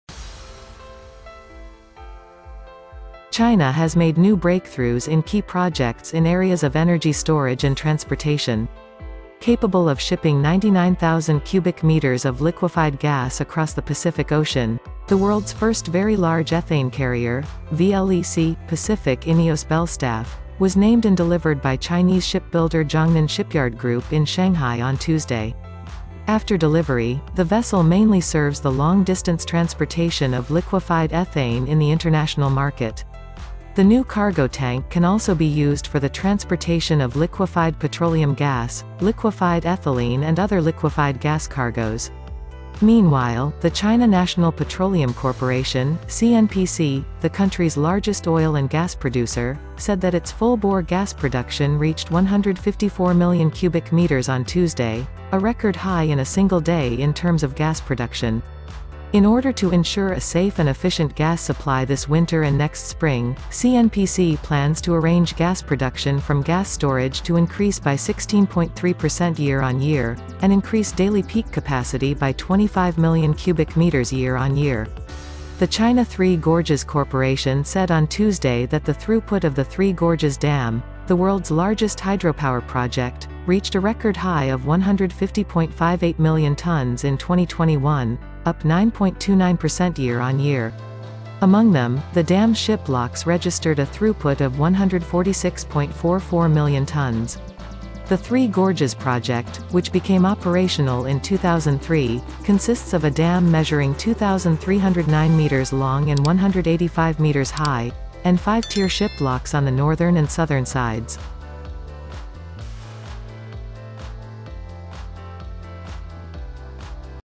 China makes new breakthroughs in energy storage and transportation | 中国在储能、运输领域取得新突破
China has made new breakthroughs in key projects in areas of energy storage and transportation.

Capable of shipping 99,000 cubic meters of liquefied gas across the Pacific Ocean, the world's first very large ethane carrier (VLEC), "Pacific Ineos Belstaff," was named and delivered by Chinese shipbuilder Jiangnan Shipyard Group in Shanghai on Tuesday.

As the largest carrier for ethane in the world by far, the vessel features the Type-B cargo quartet containment system "BrilliancE" developed by Jiangnan Shipyard. B stands for Type-B and E stands for ethane and ethylene.

After delivery, the vessel mainly serves the long-distance transportation of liquefied ethane in the international market. The new cargo tank can also be used for the transportation of liquefied petroleum gas, liquefied ethylene and other liquefied gas cargoes.

Meanwhile, the China National Petroleum Corporation (CNPC), the country's largest oil and gas producer, said that its full-bore gas production reached 154 million cubic meters on Tuesday, a record high in a single day in terms of gas production.

In order to ensure a safe and efficient gas supply this winter and next spring, CNPC plans to arrange gas production from gas storage to increase by 16.3 percent year on year, and increase daily peak capacity by 25 million cubic meters year on year.

The China Three Gorges Corporation said on Tuesday that the throughput of the Three Gorges Dam, the world's largest hydropower project, reached a record high of 150.58 million tonnes in 2021, up 9.29 percent year on year. Among them, the dam's ship locks registered a throughput of 146.44 million tonnes.

The Three Gorges project, which became operational in 2003, consists of a dam measuring 2,309 meters long and 185 meters high, and five-tier ship locks on the northern and southern sides.

中国在储能和交通领域的重点项目上取得了新的突破。

世界上第一艘超大型乙烷运输船（VLEC）“Pacific Ineos Belstaff”于周二在上海由中国造船厂江南造船集团命名并交付，能够跨越太平洋运送 99,000 立方米的液化气。

作为目前世界上最大的乙烷运输船，该船采用江南造船厂开发的B型货物四方围护系统“BrilliancE”。 B代表B型，E代表乙烷和乙烯。

该船交付后主要服务于国际市场液化乙烷的长途运输。新货舱还可用于运输液化石油气、液化乙烯等液化气货物。

与此同时，中国最大的石油和天然气生产商中国石油天然气集团公司（CNPC）周二表示，其全口径天然气产量达到 1.54 亿立方米，创下单日天然气产量新高。

为保障今冬明春安全高效供气，中石油拟安排储气库产气量同比增长16.3%，日高峰产能同比增加2500万立方米。

中国三峡集团公司周二表示，全球最大的水电项目三峡大坝2021年吞吐量创历史新高，达1.5058亿吨，同比增长9.29%。其中，大坝船闸吞吐量14644万吨。

三峡工程于2003年投入运营，由一座长2309米、高185米的大坝和南北两侧的五层船闸组成。